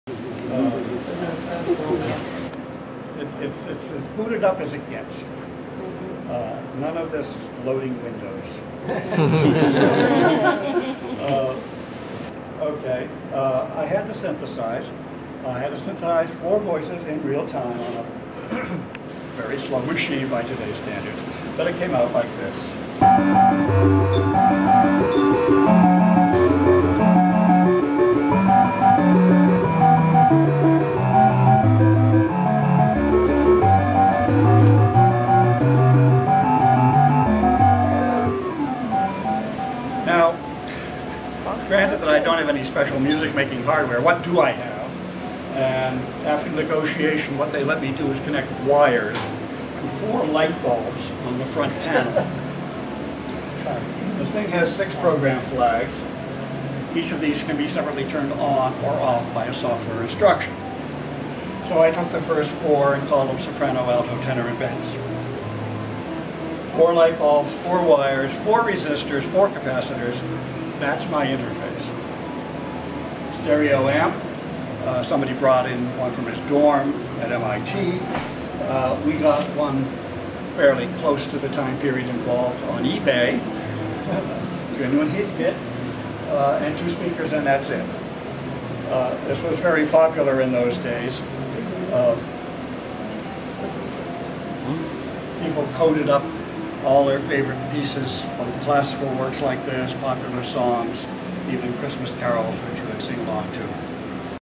Peter Samson demostrando música sintetizada en un PDP-1
Peter Samson escribió, a principios de la década del 60, un programa para componer y reproducir música en un PDP-1, y también hizo las alteraciones necesarias al computador para que pudiera emitir sonidos (algo que no era parte del diseño original).

Actualmente Peter trabaja en el Computer History Museum, donde escribió una nueva versión de su programa de música (el original se perdió) para poder reproducir los archivos de música que han perdurado hasta ahora.